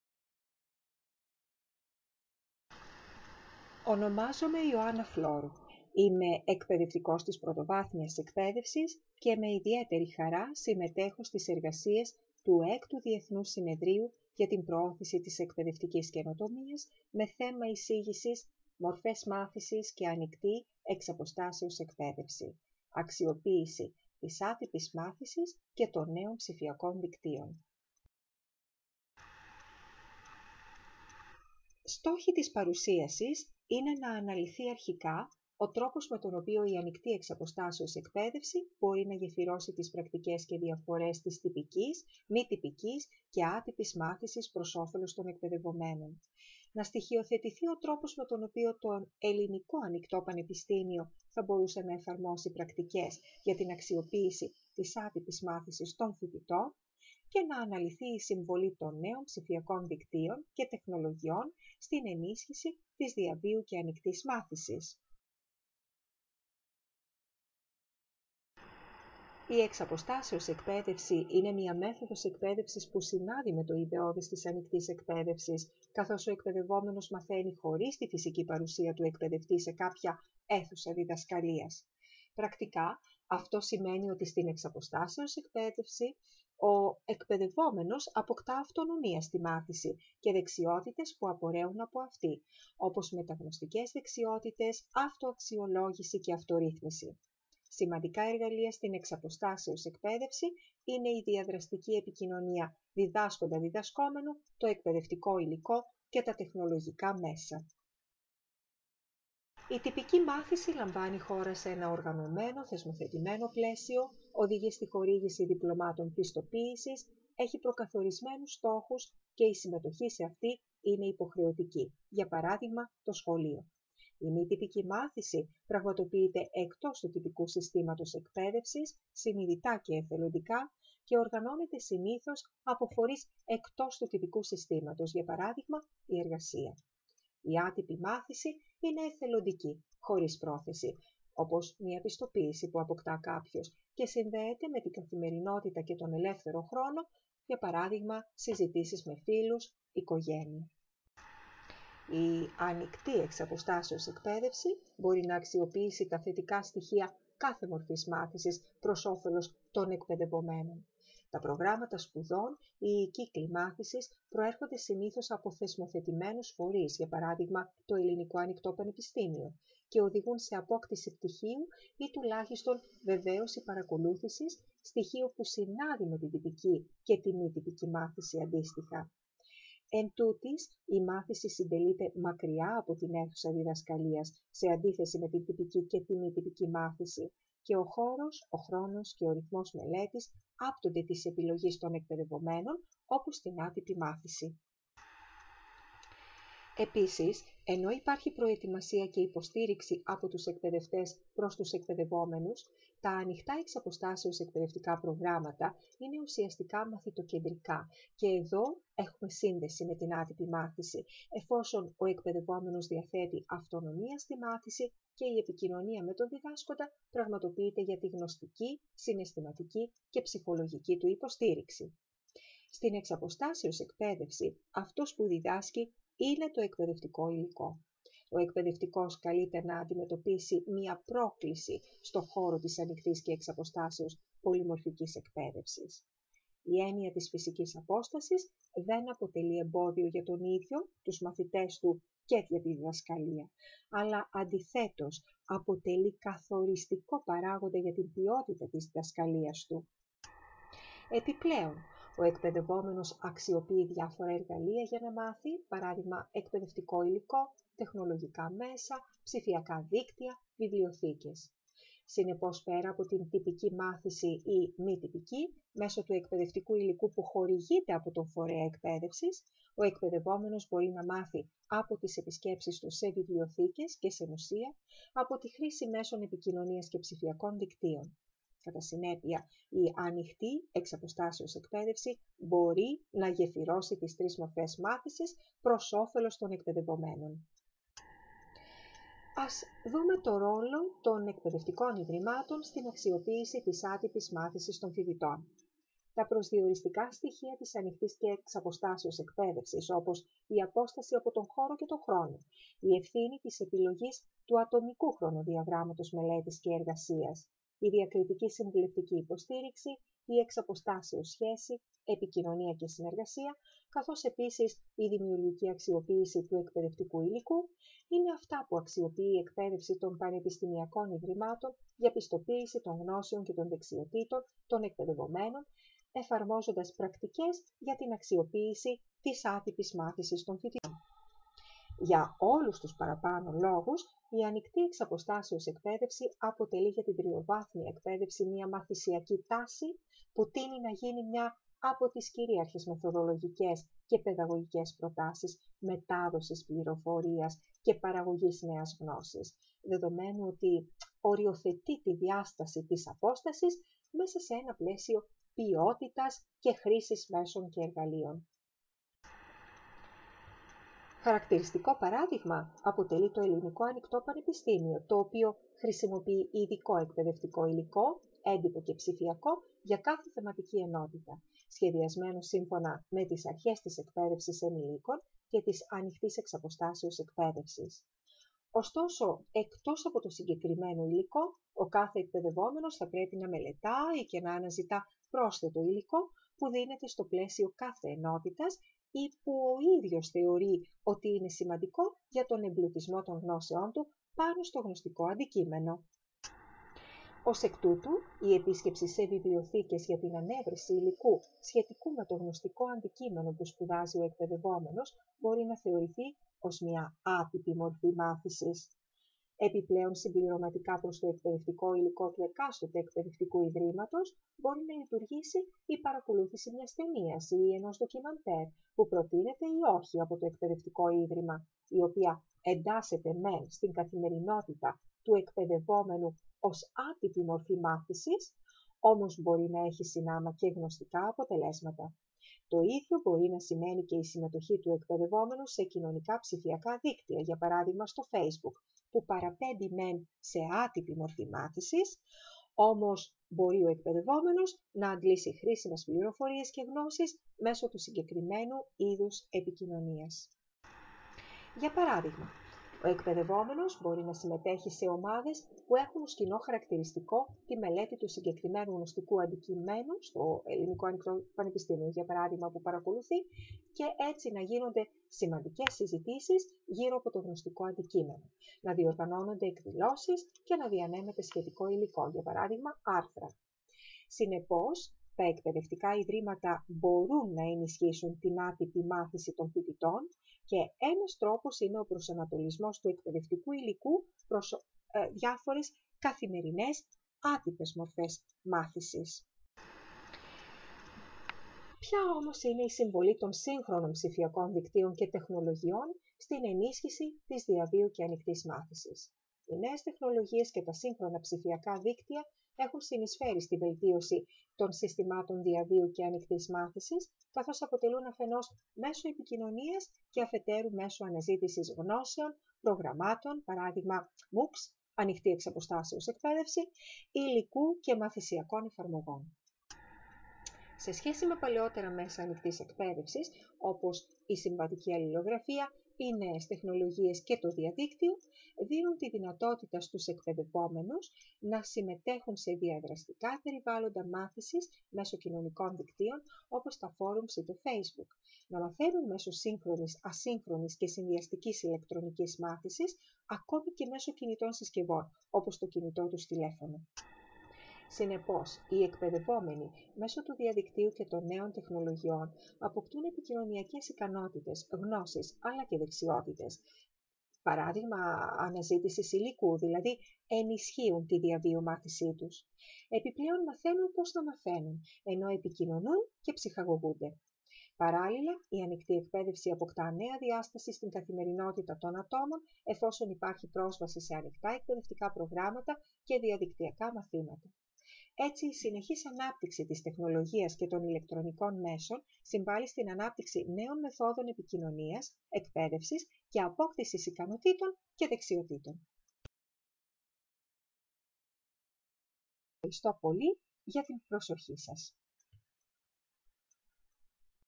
447 Μορφές μάθησης και ανοικτή και εξ αποστάσεως εκπαίδευση: αξιοποίηση της άτυπης μάθησης [...]
6th International Conference for the Promotion of Educational Innovation

Title: Μορφές  μάθησης και ανοικτή και εξ αποστάσεως εκπαίδευση:
αξιοποίηση της άτυπης μάθησης και των νέων ψηφιακών δικτύων

Φλώρου Ιωάννα
Εκπαιδευτικός, ΠΕ70, Msc στις Επιστήμες της Αγωγής